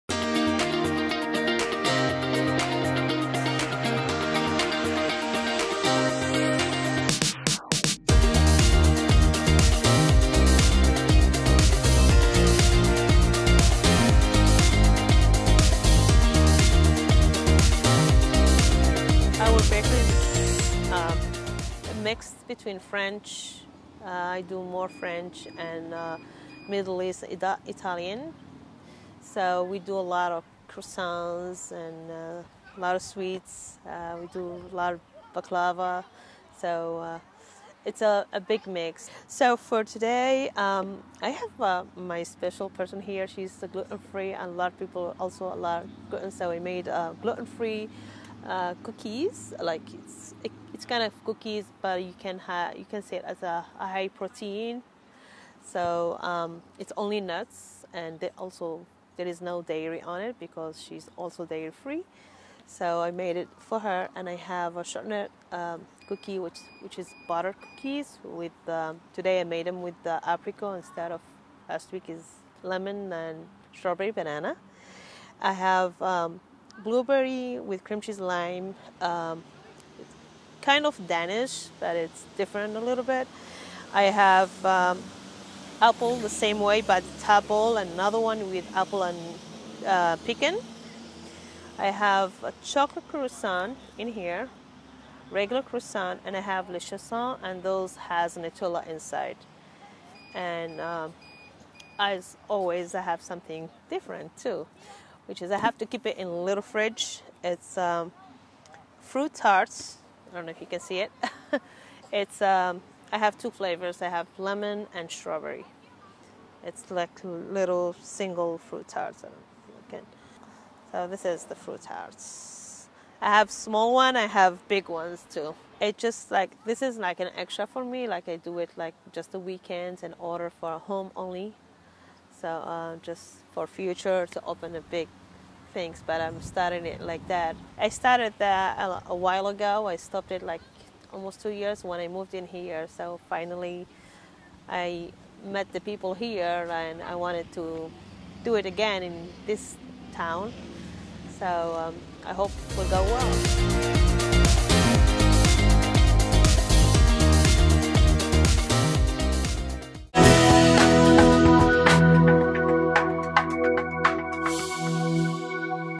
Farmers' Market Moments 2023: SSE Bakery
Ounissa of SSE Together bakery in Templeton shows us her delicious cookies, croissants and some tasty surprises in the cooler. Alert: Gluten-free cookies!